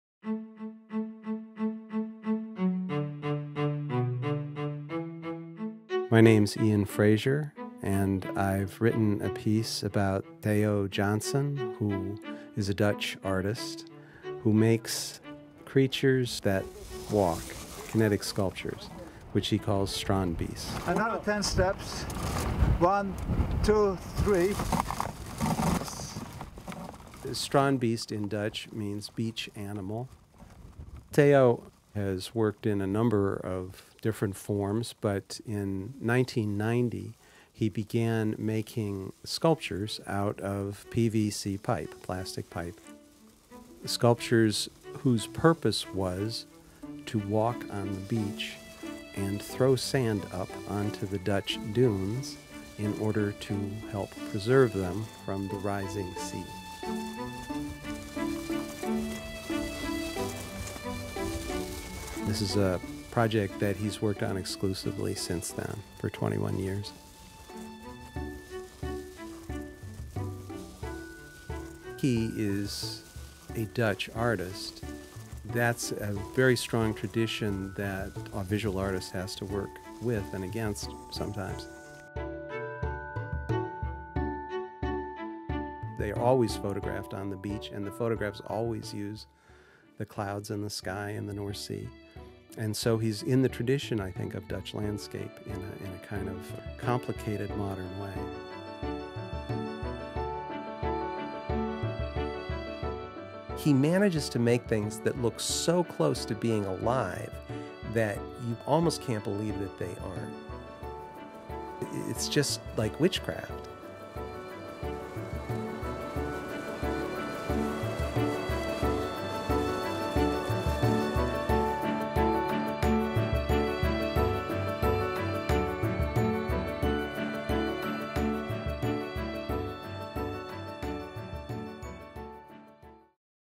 Theo Jansen’s Wind-Powered Sculptures | The New Yorker
Theo Jansen’s Strandbeests, wind-powered sculptures that walk on the beach.